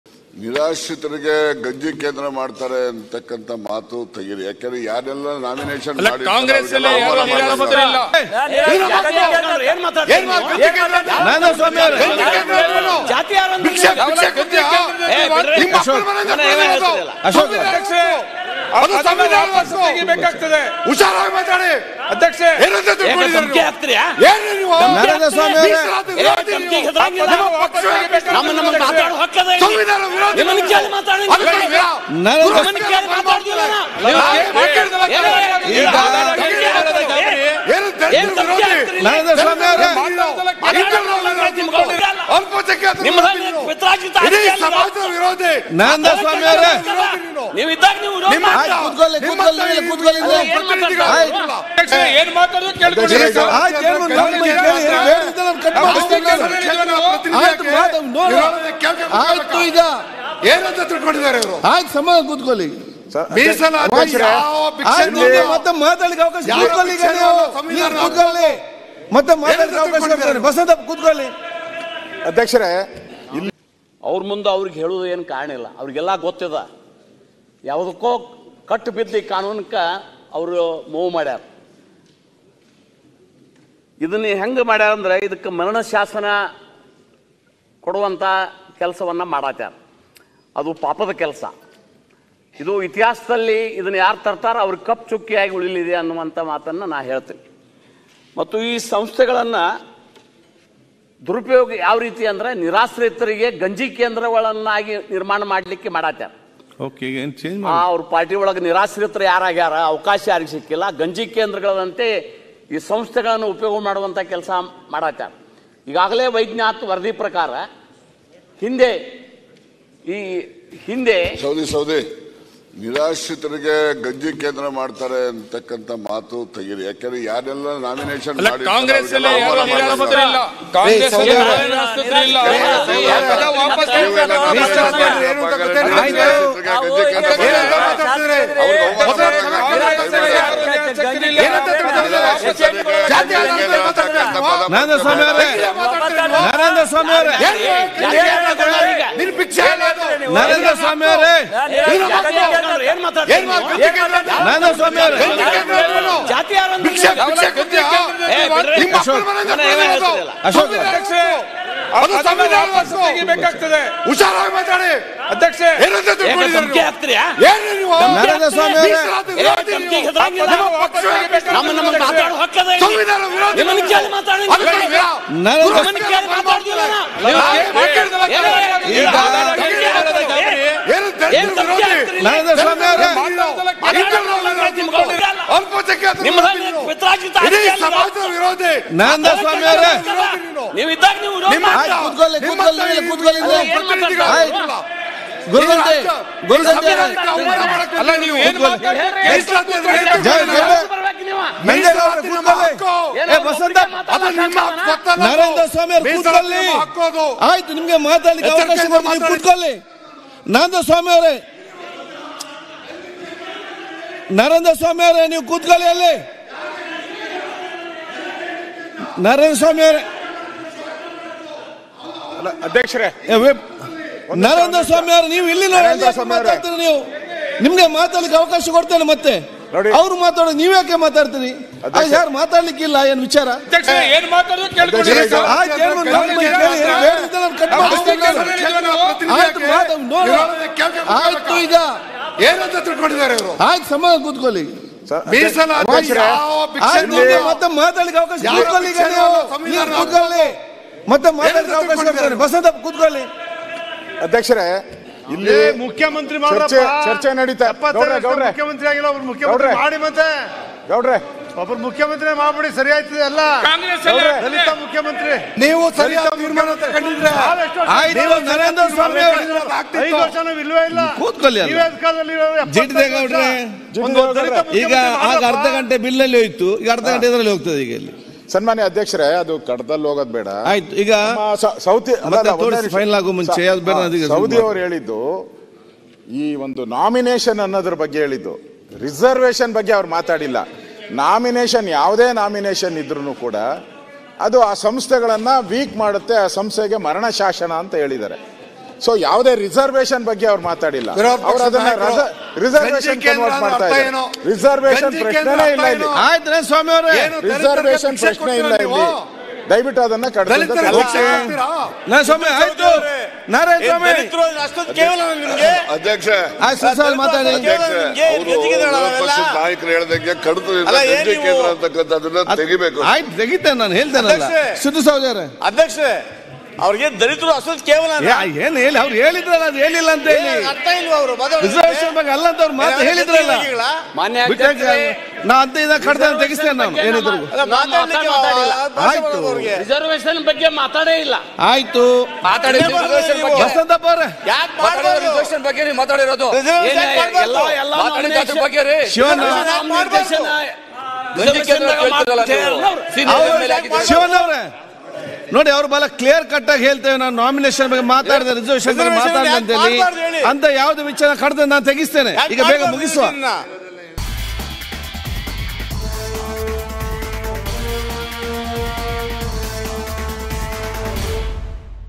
Karnataka Assembly Session | ನರೇಂದ್ರಸ್ವಾಮಿ ವಿರುದ್ಧ ಸಿದ್ದು ಸವದಿ ಗರಂ ..! | Suddiyaana
ನರೇಂದ್ರಸ್ವಾಮಿ ವಿರುದ್ಧ ಸಿದ್ದು ಸವದಿ ಗರಂ ಆಗಿದ್ದಾರೆ. ಭಿಕ್ಷೆ ಭಿಕ್ಷೆ ಕೊಡ್ತಿಯಾ? ನಿಮ್ಮ ಅಪ್ಪನ ಮನೆ ಹಕ್ಕು ಅಲ್ಲ, ಸಂವಿಧಾನದ ಹಕ್ಕು ಅಂತಾ ಹೇಳಿದ್ದಾರೆ.